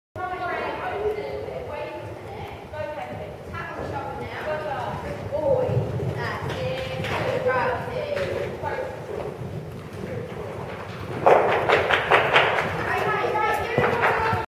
katie, lauren then georgia juming a cross
we all poped over the last jump before we finished the lesson xxx